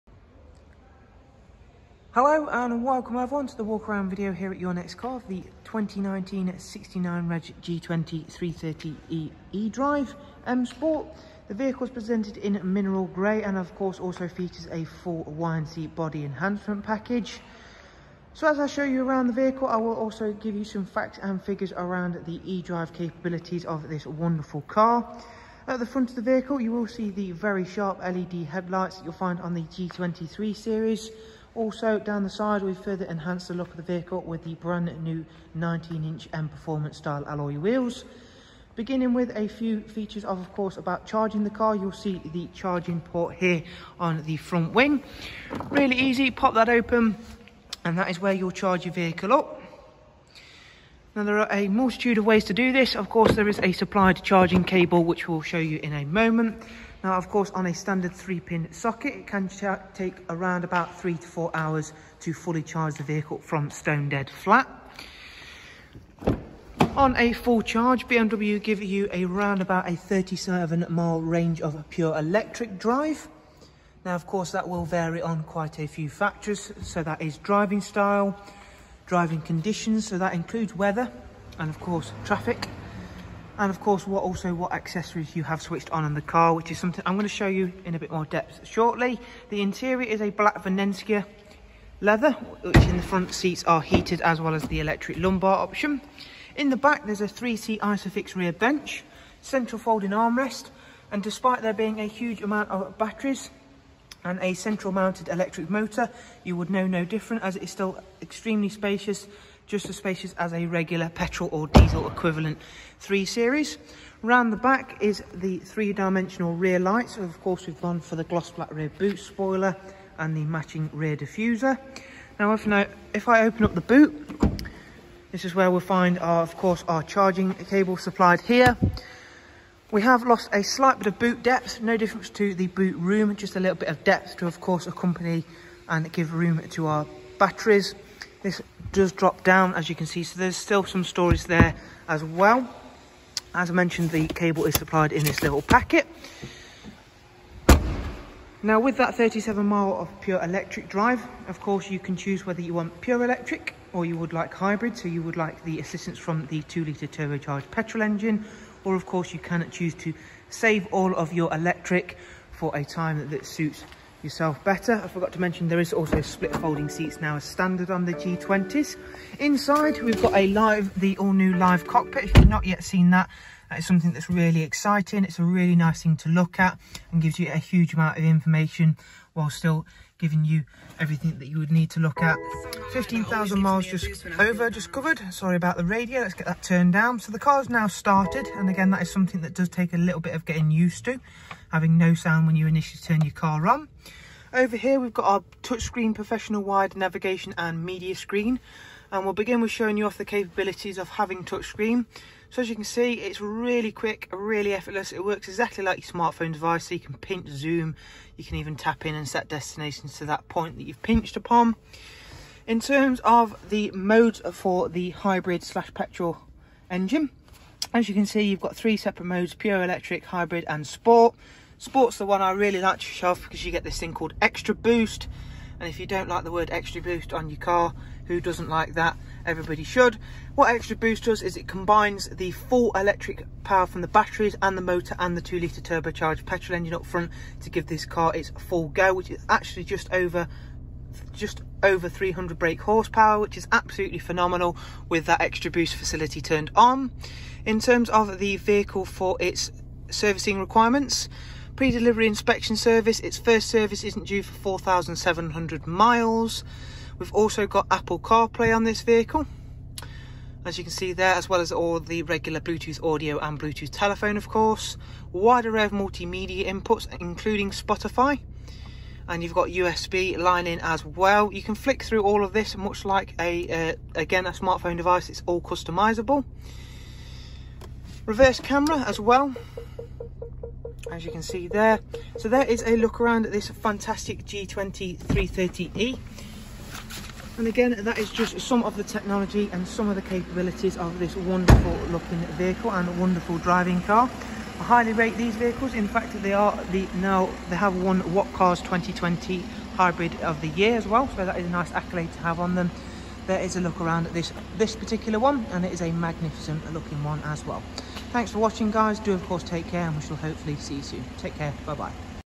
(OU69NVE) NEW @ YNC G20 330e EDRIVE M SPORT!! ( Stunning Hybrid 3 Series!!)
- 2019 69 G20 330E M Sport
- YNC Mperformance Kit
- 19' Alloy Wheels
- Rev Cam
- Heated Leather
- 37 Mile Pure E Range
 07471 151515